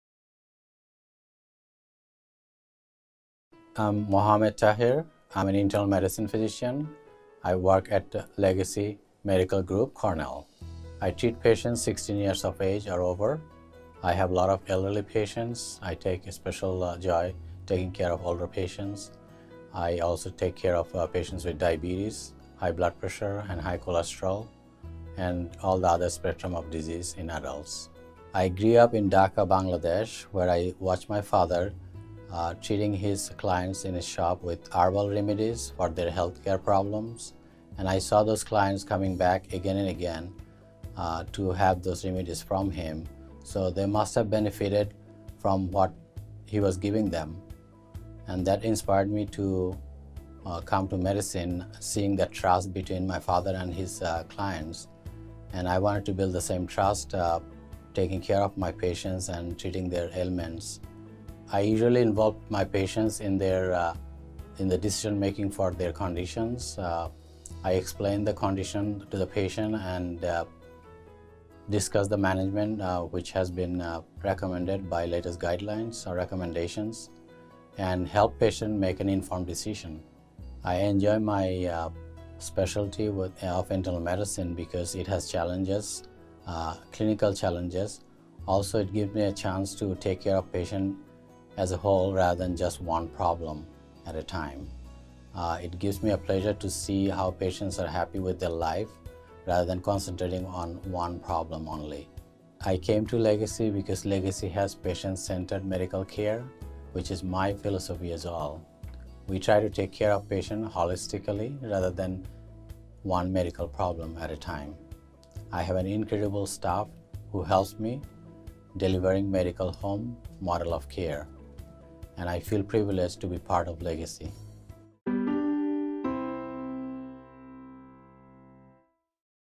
Mohammed A. Taher, MD, FACP
Dr. Taher practices evidence based medicine, with experience and special interest in diabetes, heart disease, and care for the elderly. He strives for disease prevention and the promotion of a healthy lifestyle. With more than 20 years of experience in this specialty, Dr. Taher has been recognized by the National Committee for Quality Assurance through the Diabetes Physician Recognition Program, served on various committees in hospitals from Pendleton to Hillsboro, and has taught Medical Students in the United Kingdom and Pennsylvania.